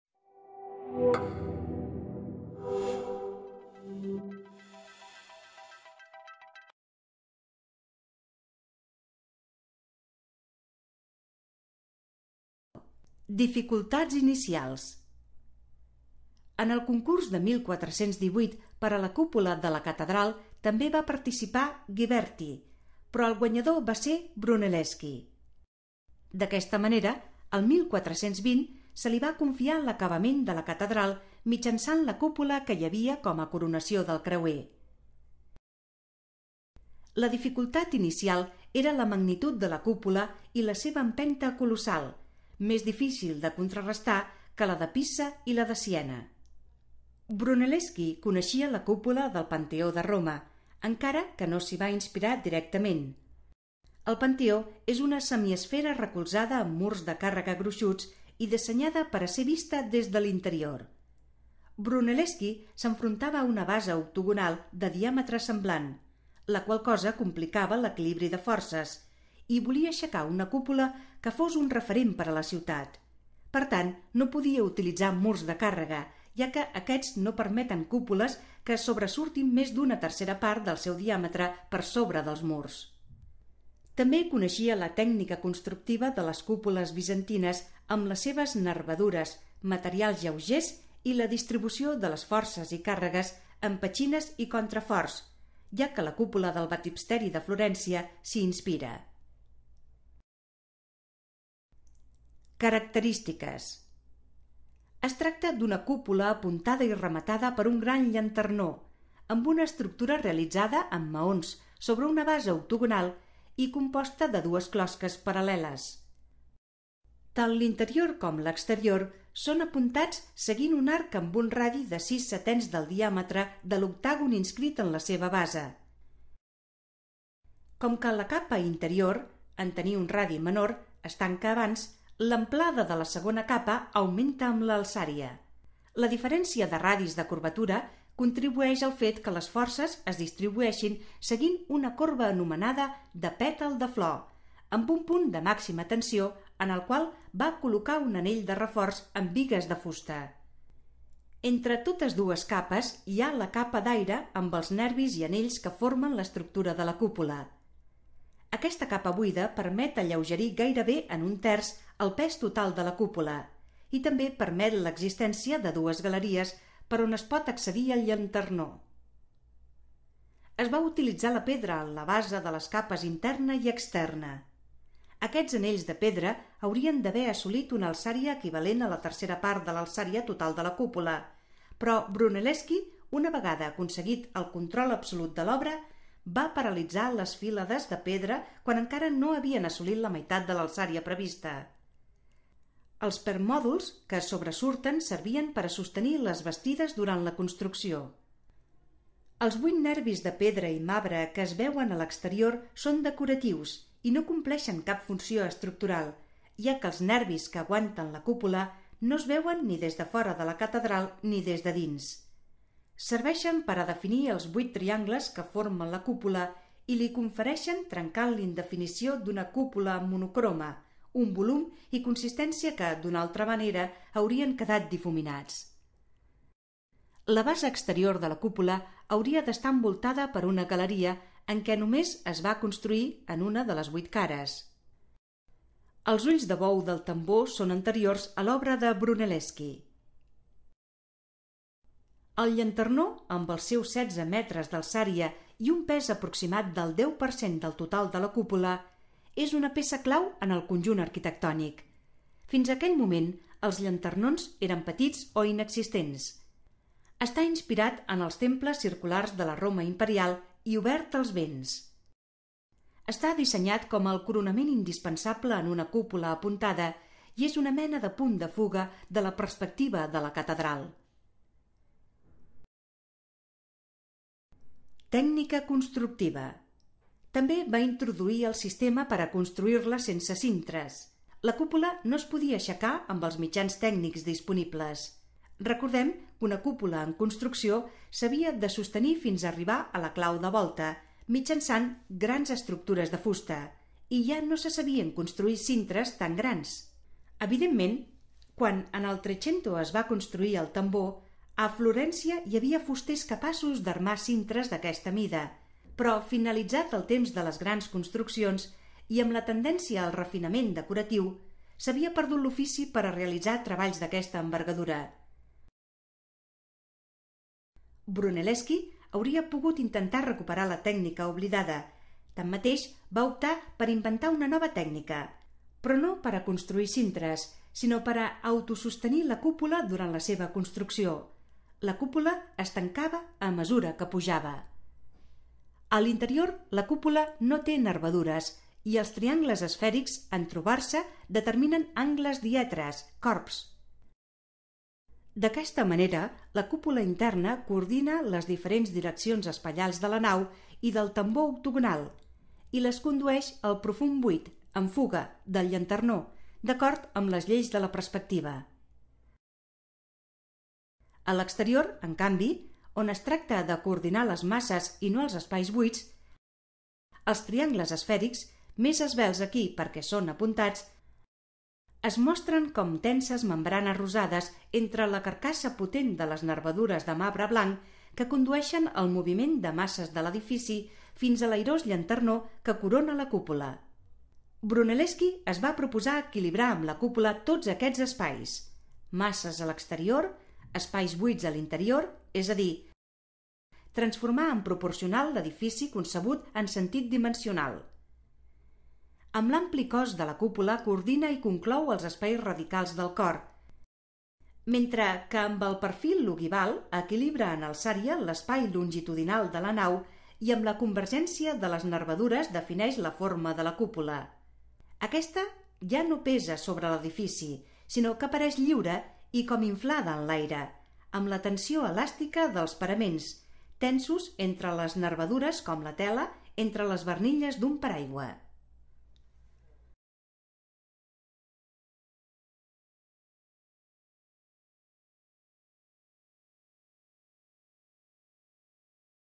Resum d'Història de l'Art. 19. Renaixement: Brunelleschi: cúpula del Duomo de Florència
Com va poder un orfebre, cèlebre pel seu mal caràcter i sense formació acadèmica com a arquitecte, construir una de les joies més belles del Renaixement italià?

Projectada i construïda per Filippo Brunelleschi (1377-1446), es considera que amb ella es va iniciar el Renaixement italià i florentí en arquitectura.

Quins eren els problemes que plantejava la construcció d’aquesta cúpula? Ningú semblava capaç de concebre un projecte viable d'una cúpula de gairebé 50 metres d'ample, sobretot perquè calia començar a edificar-la a 55 metres d'altura, sobre els murs ja existents. A més, els projectes de construcció previstos evitaven els arcbotants i els arcs ogivals propis de l'estil gòtic tradicional. Podria una cúpula de desenes de milers de tones sostenir-se sense cap d'aquests elements? Hi hauria prou fusta en tota la Toscana per les bastides i cintres necessaris per construir la cúpula? ¿Es podria aixecar l'estructura sobre la planta octogonal imposada pels murs existents sense que s'ensorrés pel centre?

En el concurs projectat per a la seva construcció, Brunelleschi, va prometre construir no una cúpula sinó dues, una paral·lela a l'altra i connectades entre si, sense aixecar complicades i costoses bastides.

La seva cúpula consistiria en dos casquets concèntrics: un d'interior, visible des de dins de la catedral, allotjat dins la cúpula exterior, més ampla i més gran. Per contrarestar l’empenta lateral (la pressió cap a fora creada pel pes d'una gran estructura, que es pogués esquerdar o causar el seu enfonsament), reforçaria els murs anellant-los amb cèrcols de pedra, ferro i fusta. La dificultat inicial radicava en la magnitud de la cúpula i la seva empenta colossal.

El vídeo explica aquestes dificultats i descriu les característiques de la cúpula així com la tècnica constructiva utilitzada.